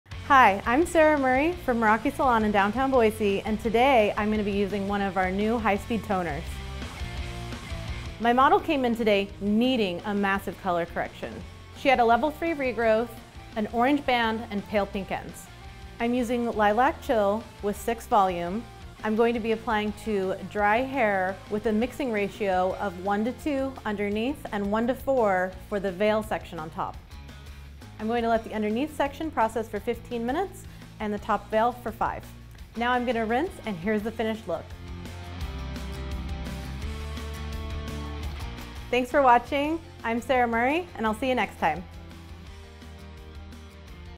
COLOR CORRECTION | Pulp Riot High Speed Toner in Lilac Chill
Watch Pulp Riot Educator @hairdesignsara take Lilac Chill High Speed Toner to the max with this bold, blonde transformation.

Lilac Chill is a violet-based toner that adds cool, violet tones to hair while eliminating unwanted yellow undertones.